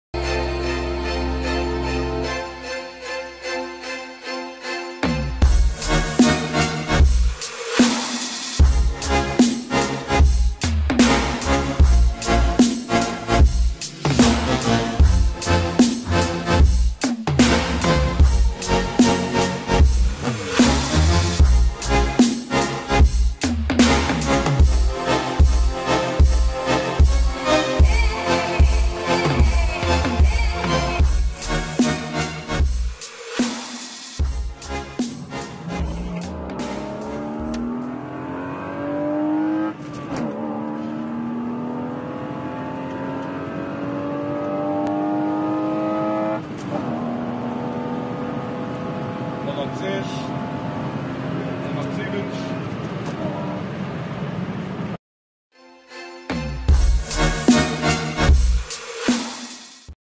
BMW E36 325 M50B25 onboard
A little run with my e36 325 M50B25 with Vanos and only K&N induction Kit.